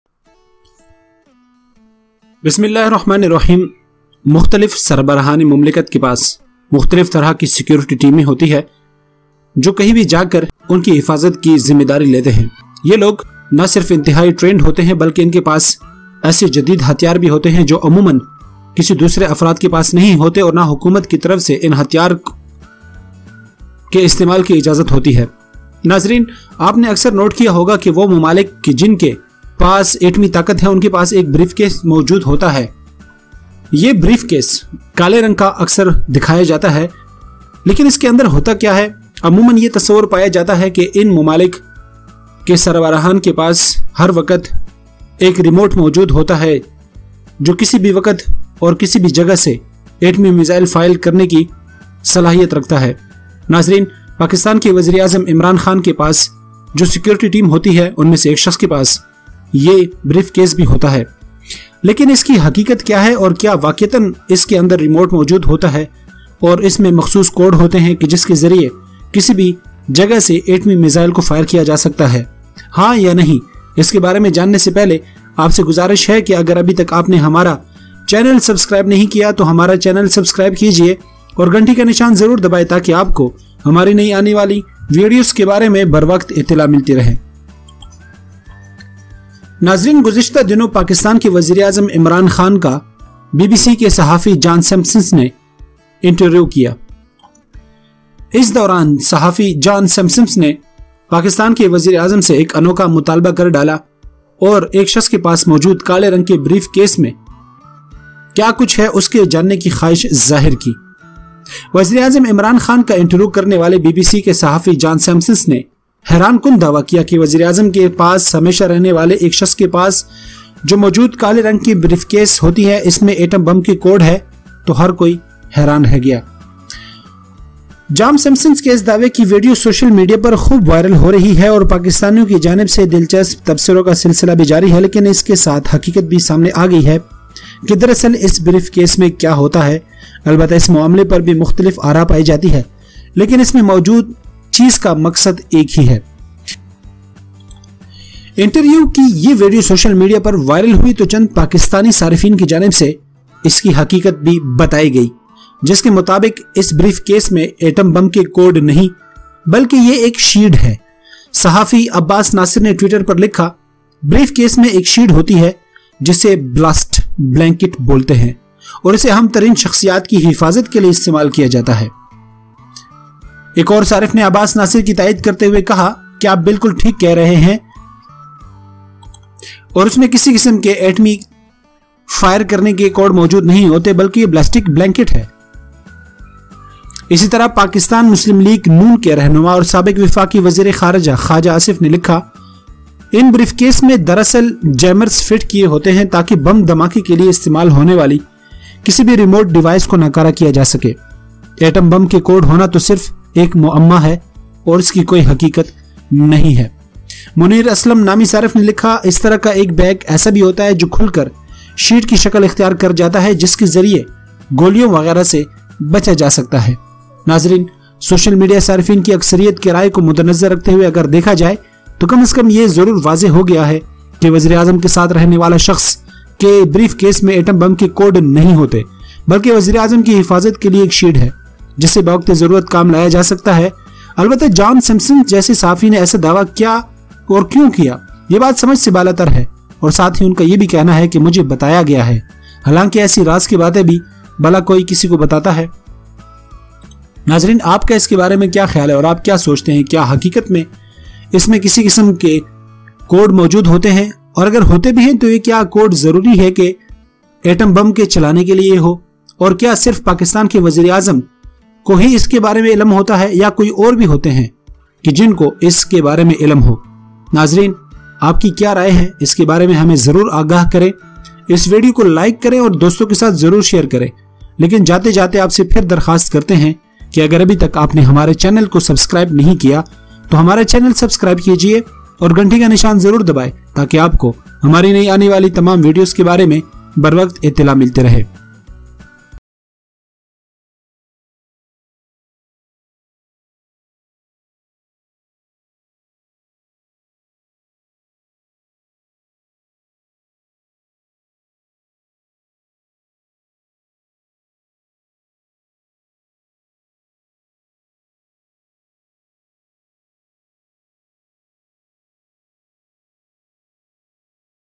Mysteries Black Bag follows Prime minister of Pakistan
BBC Journalist John Simpson, who interviewed Prime Minister Imran Khan, surprisingly claimed that a person living with the prime minister always has the code of atom bomb in a black rifle case, so everyone was shocked.Today we will tell you the truth about this claim, Don't Forget to see our video complete from start to end Thanks.